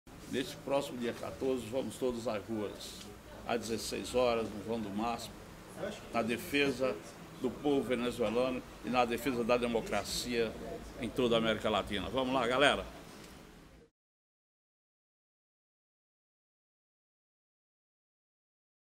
Gegê, com a Venezuela, ombro a ombro!
Luiz Gonzaga da Silva, o Gegê, do Movimento de Moradia no Centro (MMC) e da Central de Movimentos Populares (CMP) em apoio ao povo venezuelano.

Com a Venezuela, ombro a ombro!

14/05/14 -- 16h -- Mobilização no MASP -- Avenida Paulista

15/05/14 -- 19h -- Lançamento do Filme "Meu amigo Hugo Chavez" de Oliver Stone - na Sala dos Estudantes da Faculdade de Direito do Largo São Francisco (USP)